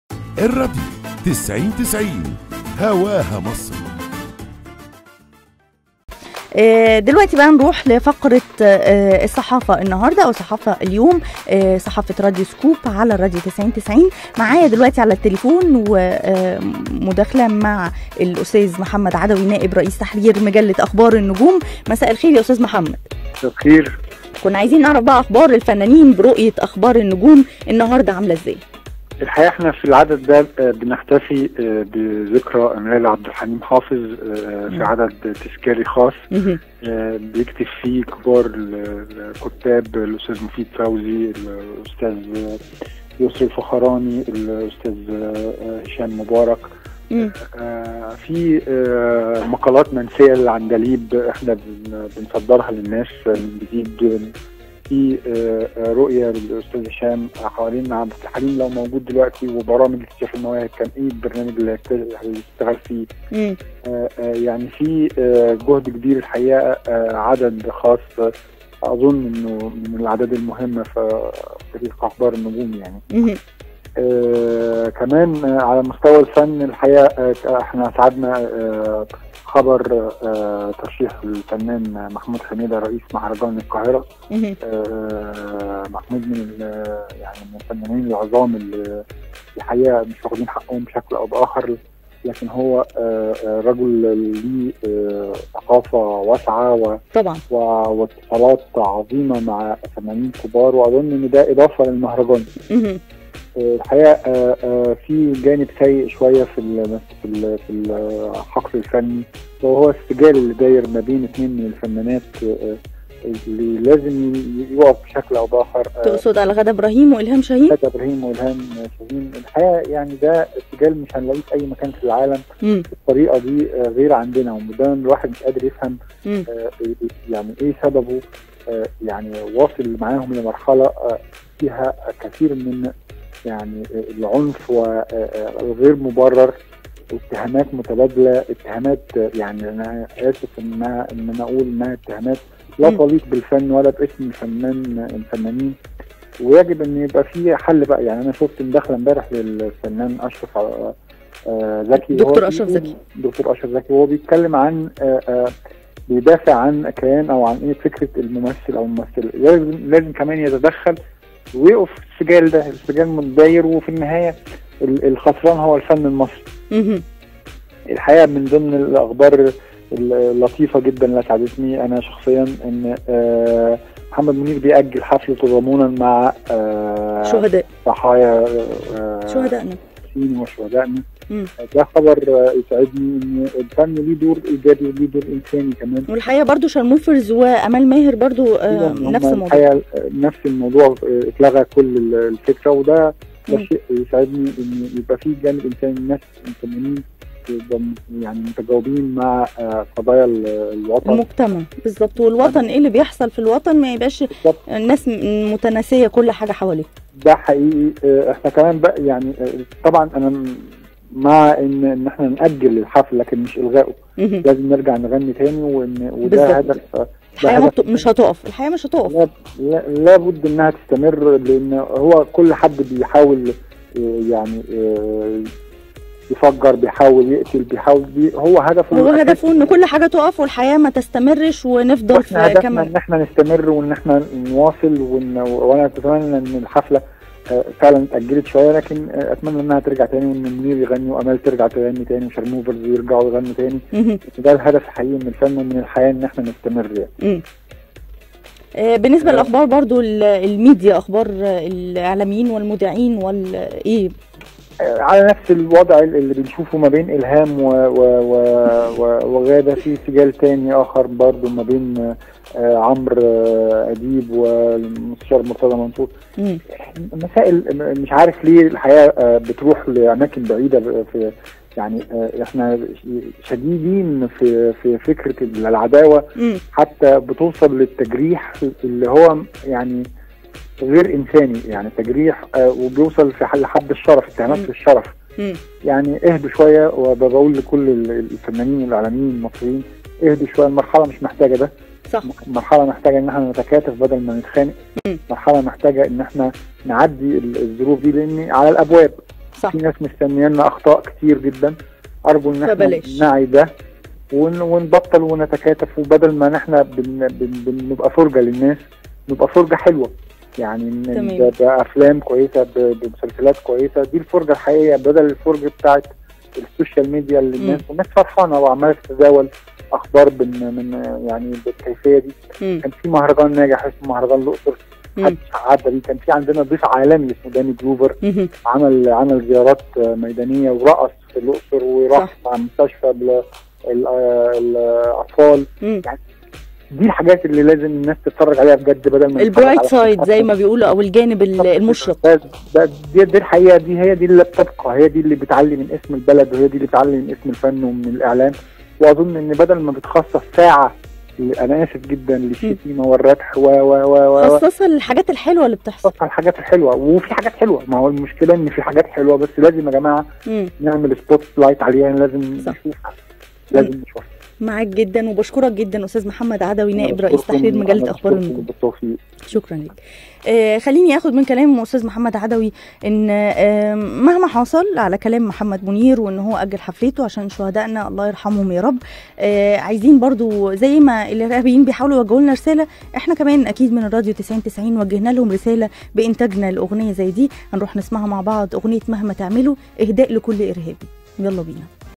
أخبار : ذكرى العندليب عبد الحليم - محمود حميدة في مهرجان القاهرة - تأجيل حفلة منير- علي الراديو 9090
راديو سكوب - غدير حسان
أكد محمد عدوى نائب رئيس تحرير مجلة اخبار النجوم ان العدد القادم من المجلة هو عدد خاص لذكرى العندليب عبدالحليم حافظ ويتناول العديد من الاسرار فى حياة العندليب
واضاف خلال مداخلة هاتفية على الراديو 9090 مع غدير حسان من الاخبار الت أسعدتنى اختيار الفنان محمود حميدة رئيس شرفى لمهرجان القاهرة السينمائى وايضا خبر تأجيل حفلة محمد منير تضامنا مع ارواح الشهداء